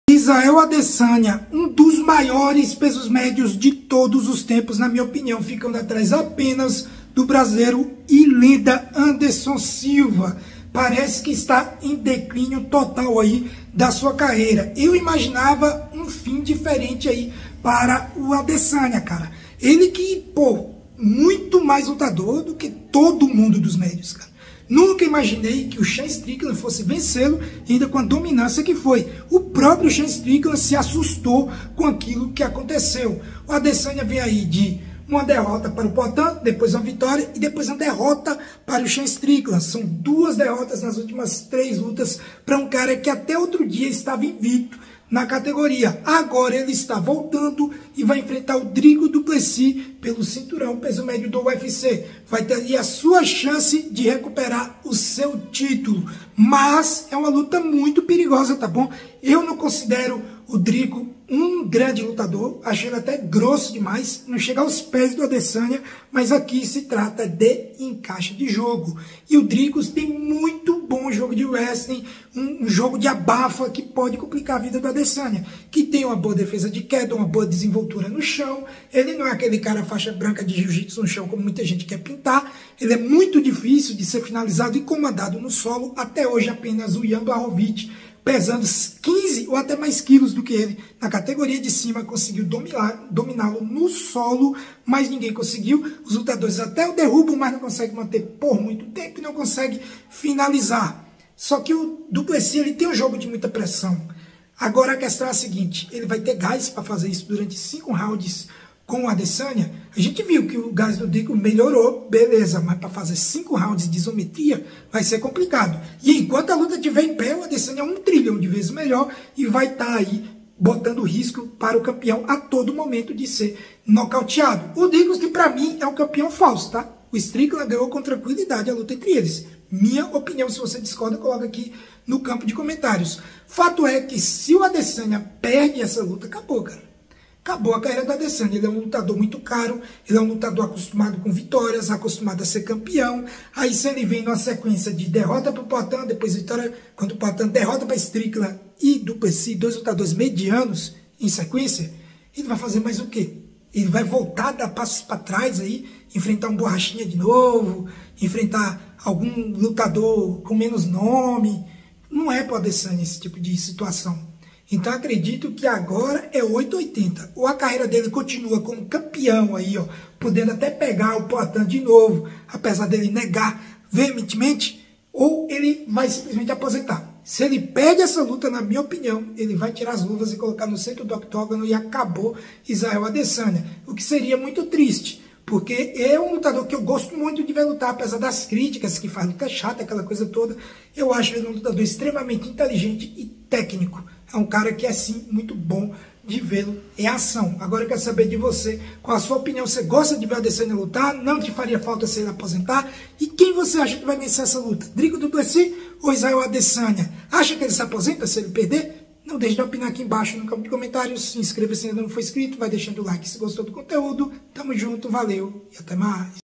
O TRISTE FIM DE ISRAEL ADESANYA
Israel adesanya é sem dúvida um dos maiores lutadores da história do MMA, mas vem passando por uma péssima fase vindo de duas derrotas e uma vitória nas últimas duas lutas onde perdeu para Alex poatan e Sean strickland, agora enfrenta o atual campeão dricus du plessis. será que adesanya aposenta em caso de nova derrota?